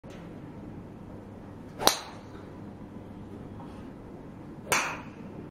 【北海道 ゴルフ】Golf ドライバーの練習
まだまだブレがありますが、

少しは良くなってきました。

トップが良いおさまりになれば、結構いい球も出てきました。